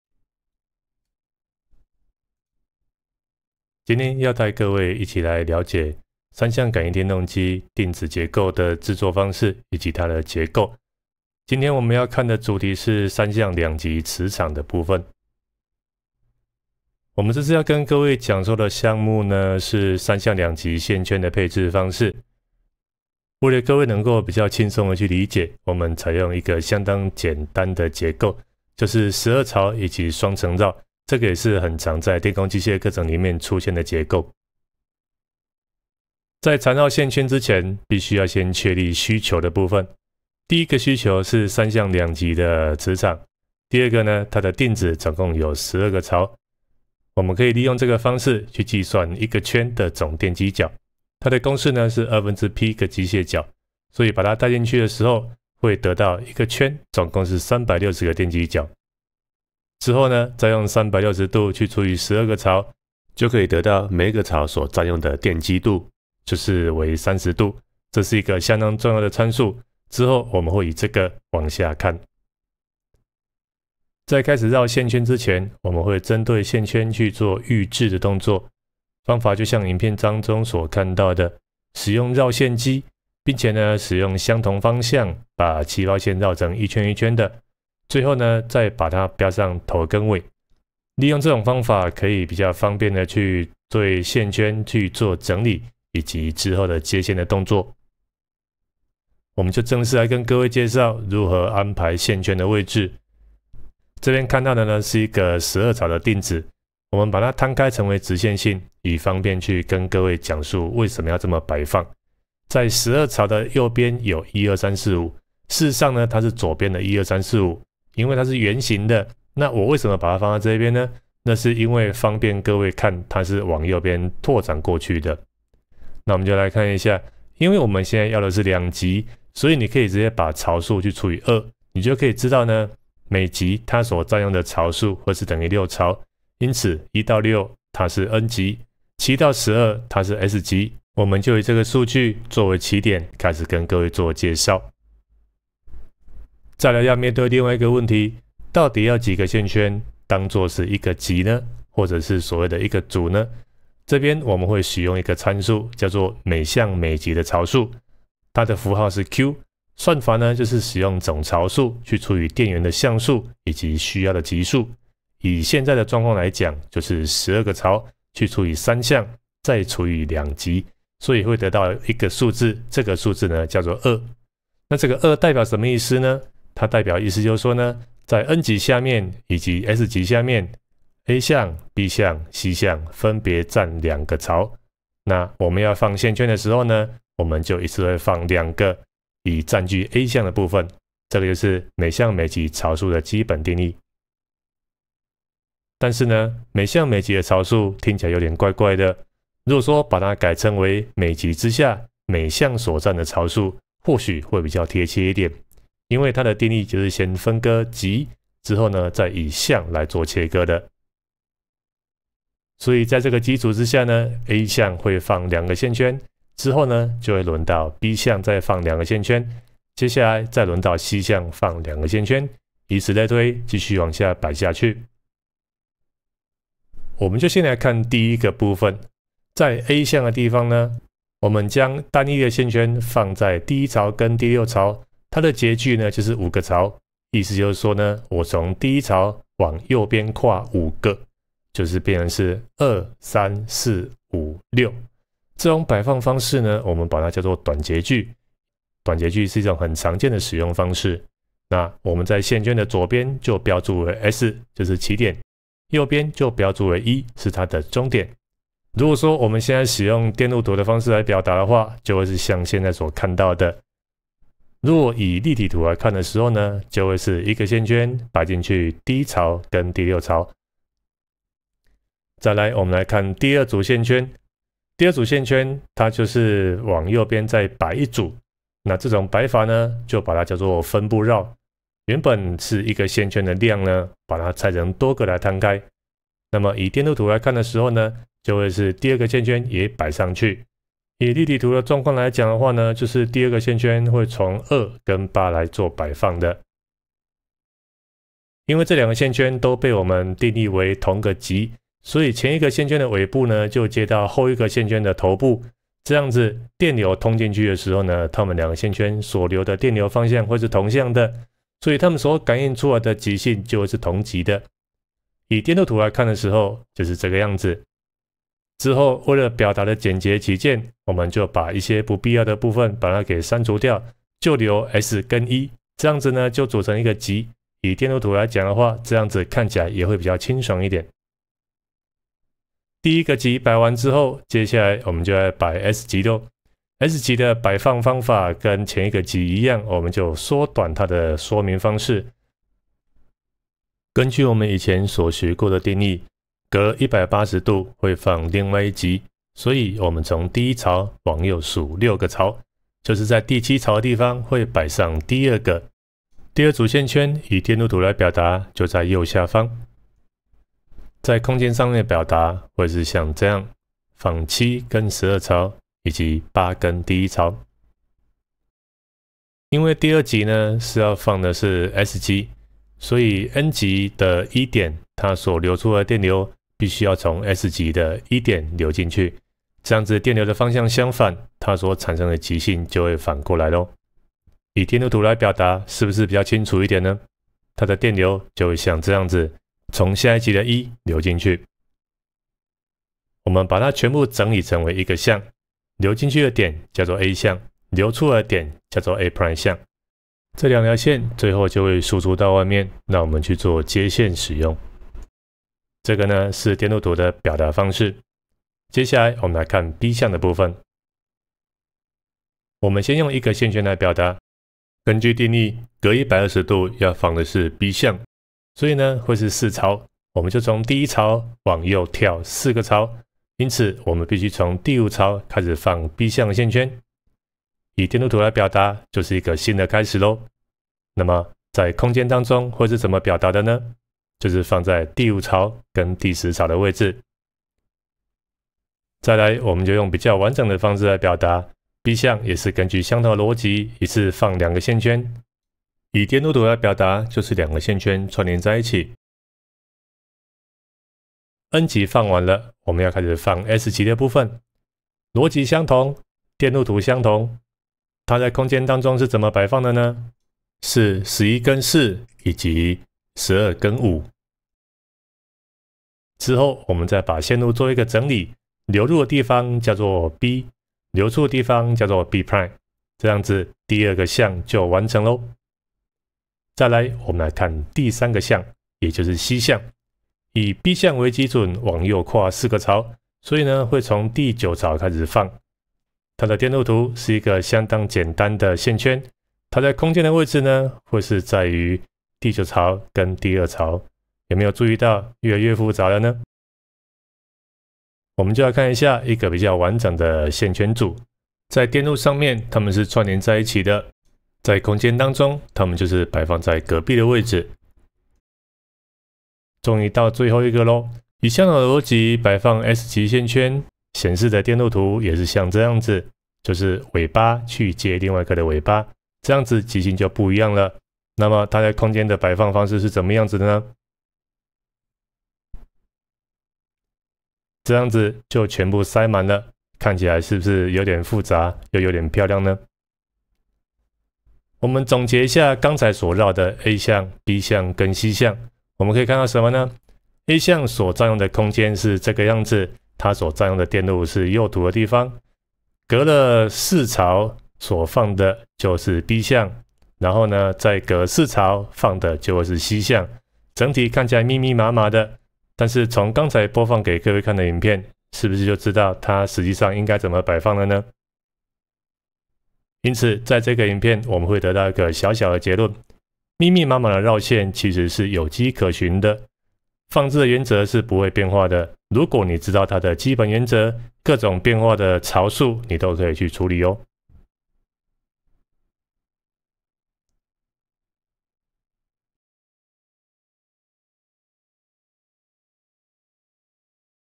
三相感應電動機，定子結構：如何繞製三相 2 極磁場
電工機械大致上有兩個學派，使用的符號不同。以本影片提到的「每相每極槽數」
本影片符號為 q， 有的課本使用的符號為 m

0:00 基本介紹
2:25 每相每極的槽數
3:45 A相繞組
6:51 B相繞組
7:56 C相繞組
9:47 三相總合